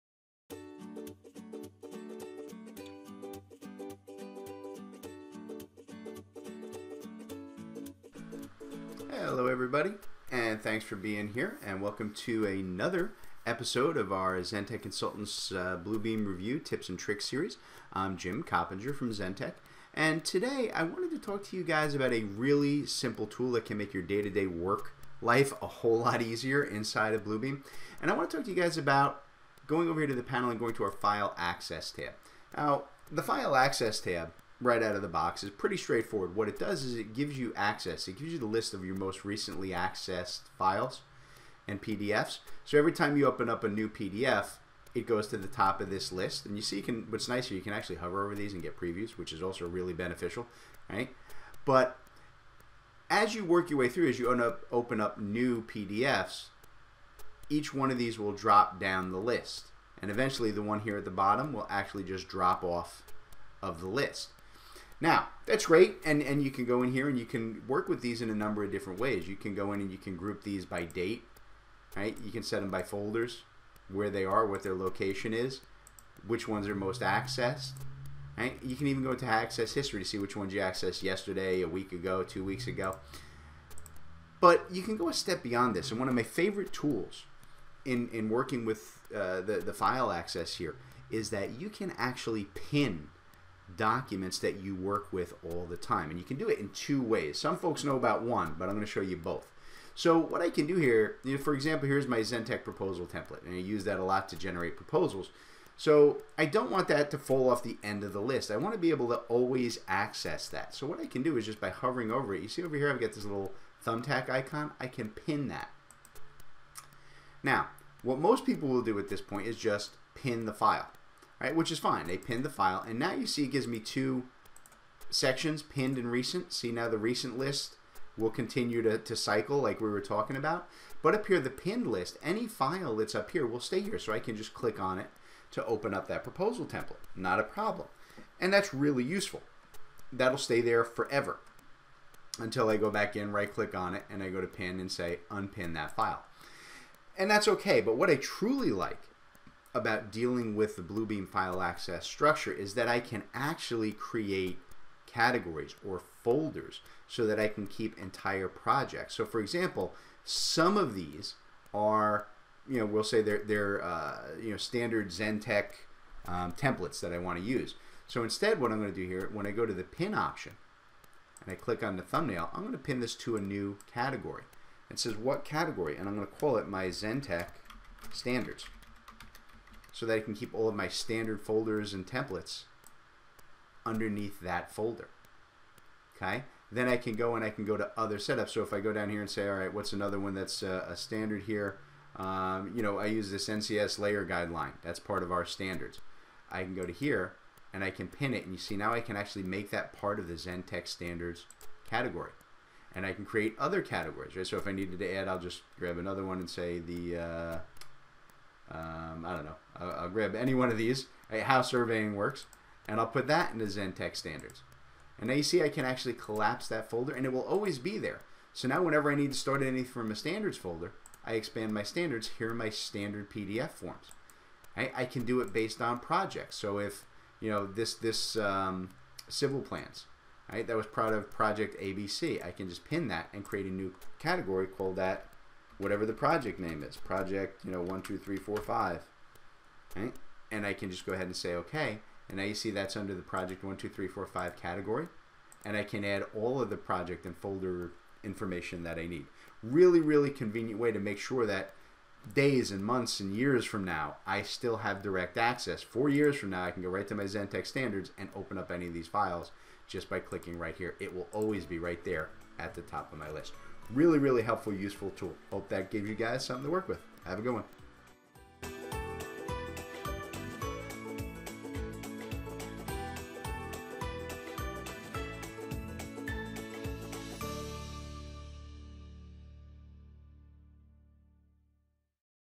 Bluebeam Revu: Accessing & Pinning Recent Files
In this tips and tricks video. we'll show you a simple tool for accessing and pinning recent files. We'll show you two ways to pin your recently accessed files so they don't drop out of the list and you can always access them quickly.

► Bluebeam Revu tool sets?
► Bluebeam Revu estimating sets?
► Bluebeam Revu Tool Chest Customization?